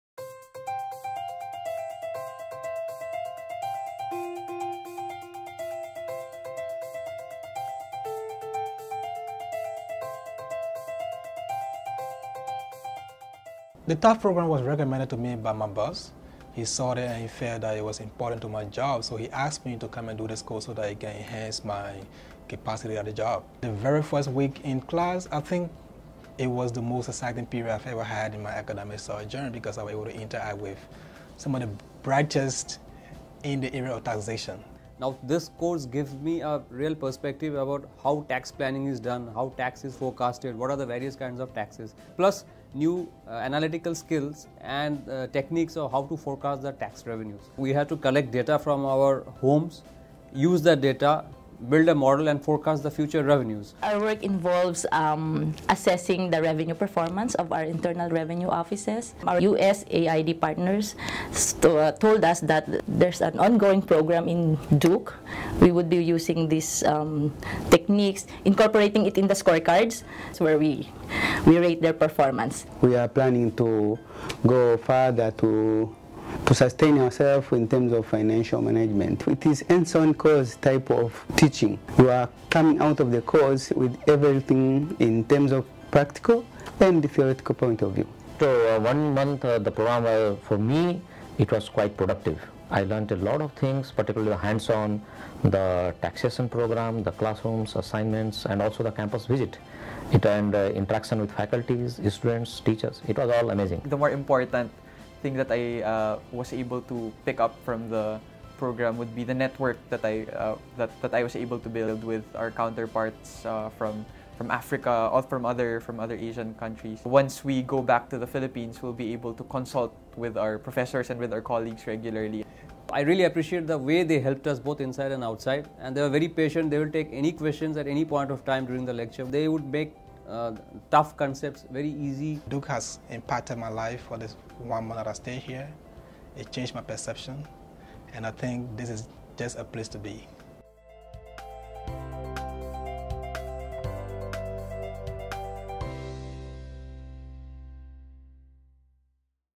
Tax Analysis and Revenue Forecasting (TARF)
DCID offers the four-week TARF program every summer to professionals interested in learning more about tax policy, revenue forecasting, statistical techniques, and computer-based revenue estimation models. Find out more about the program and the difference it makes to its alumni throughout the world.